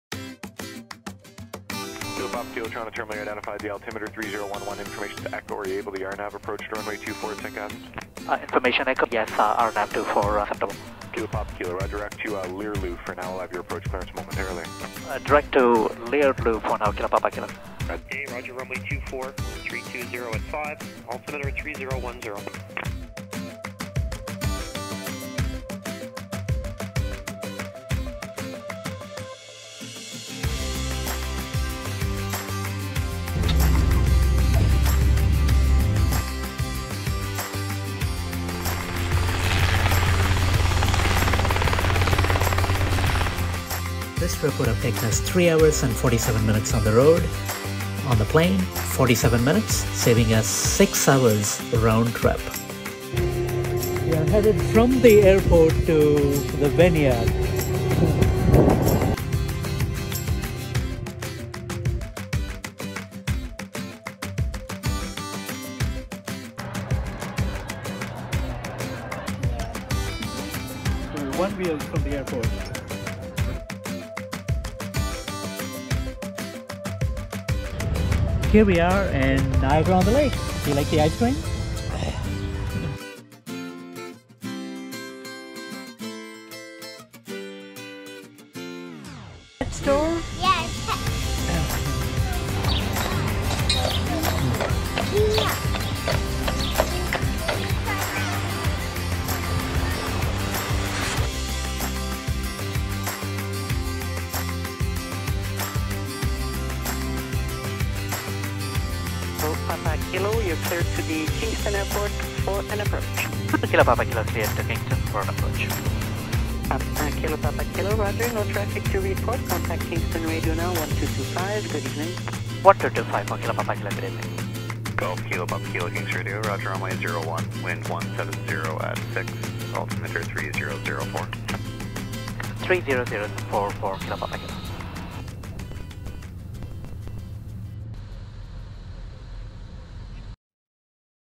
Niagara-On-The-Lake (CYSN airport) Day Trip Ideas, Saving Six Hours Round Trip on a Twin Cessna 340A
In this video we take the Twin Cessna to Niagara-On-The-Lake (nearby airport CYSN) for a day trip from Kingston, saving over six hours round trip. With the twin we are able to cross the lake. (note that I used the videos from a couple of day trips to CYSN; audio is from IFR landing on 24, the video is from VFR landing on 06)